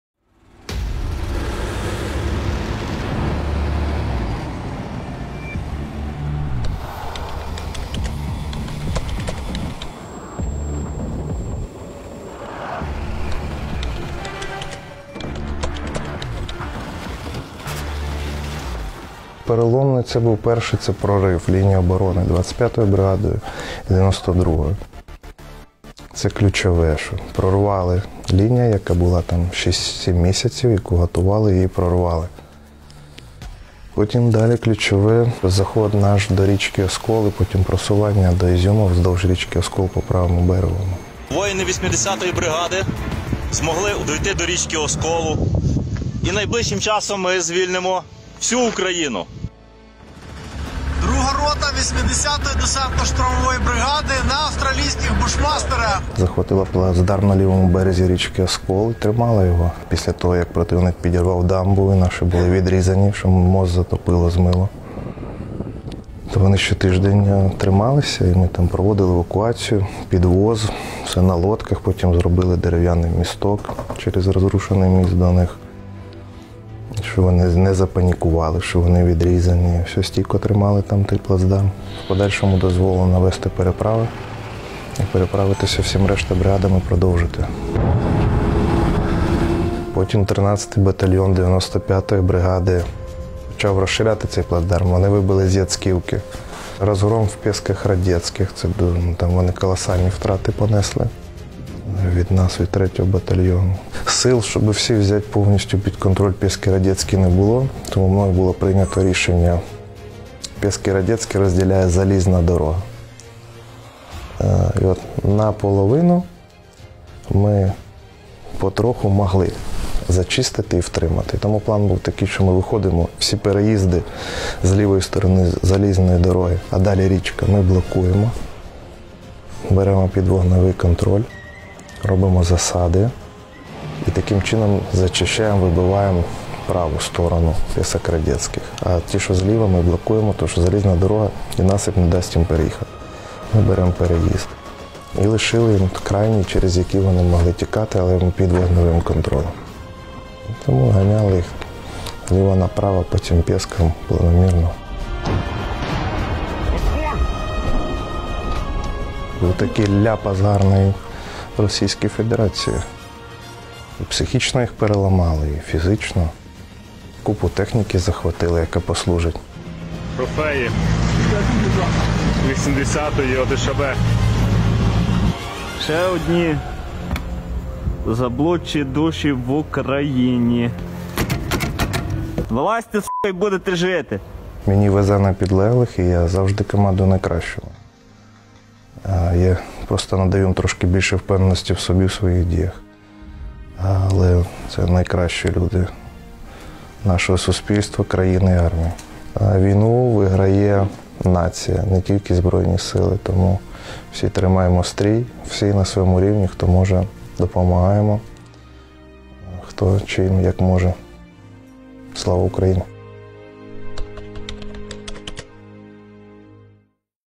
Для наближення нашої Перемоги важливі зусилля кожного: і на фронті, і в тилу
Згуртованість, сміливість, взаємодопомога на шляху до спільної мети - наша найсильніша зброя.
"Не лише Збройні Сили України, а вся держава має працювати на Перемогу. Разом повинні тримати стрій, на своєму рівні допомагати, хто чим може", - розповідає командир Павло Розлач.
Восени 2022 року батальйон під його керівництвом брав участь у операції, яка вже увійшла до історії війни, у результаті якої було визволено майже всю Харківську область.